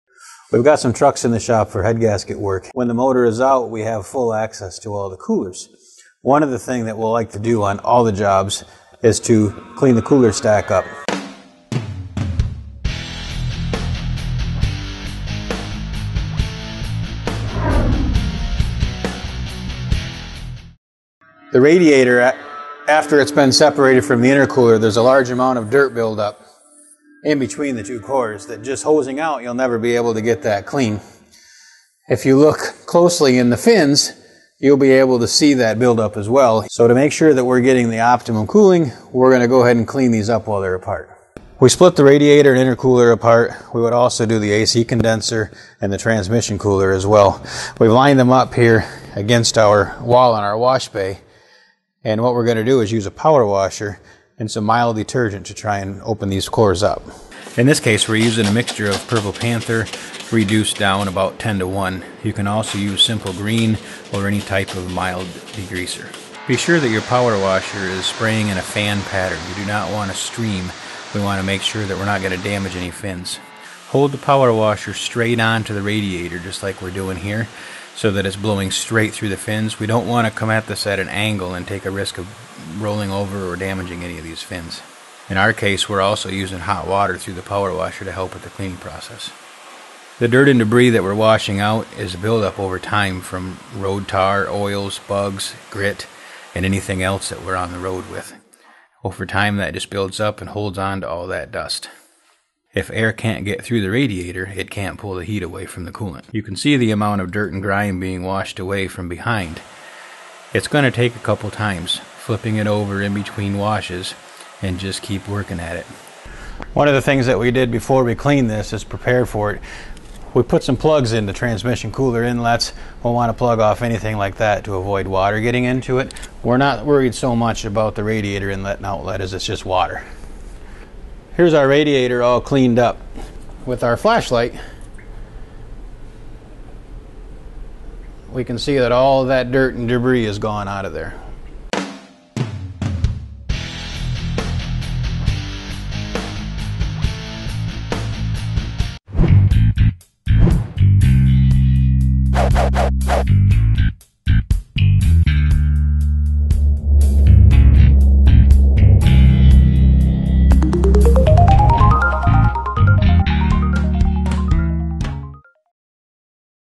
Must-Know Secrets: Duramax Truck Cooling Stack Maintenance
Your Duramax truck run hot in the summer months?  We are highlighting a common issue this week with cleaning the cooling system "stack"  The stack refers to the radiator, intercooler, air conditioning condenser and the transmission cooler mounted behind the grill.

As the truck is driven, overtime road debris, bugs, oils and tar and lots of other debris will lodge into the cooling fins of these coolers and restrict air flow.  If air can not get through the coolers, cooling capacity is greatly reduced. Here we cover the basics on cleaning those fins.

Have questions? Reach out to our technical specialists today!